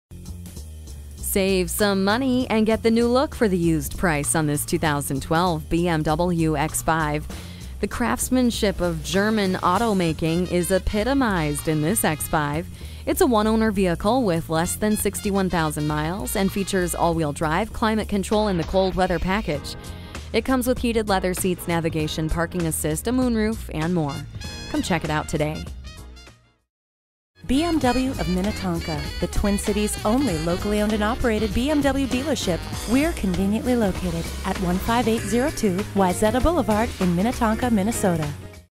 Used 2012 BMW X5 Minnetonka MN Minneapolis, MN B1161A - SOLD
Year: 2012
Make: BMW
Model: X5
Trim: AWD 4dr 35i Premium 3.0L I6
Engine: 3.0 L I6 Cyl.
Transmission: 8-Speed A/T
Color: Alpine White
Interior: Oyster
Mileage: 60865
Stock #: B1161A
VIN: 5UXZV4C50CL746215
Cold Weather Package (Heated Rear Seats, Heated Steering Wheel, Retractable Headlight Washers, and Ski Bag), Convenience Package, and Cloth. Previous owner purchased it brand new! Want to save some money? Get the NEW look for the used price on this one owner vehicle. The craftsmanship of German auto making is epitomized in the X5. Motor Trend Buyer's Guide credits X5 drives like a real Bimmer, ensuring an owner of a BMW automobile can feel right at home in a X5.

Address:
15802 Wayzata Blvd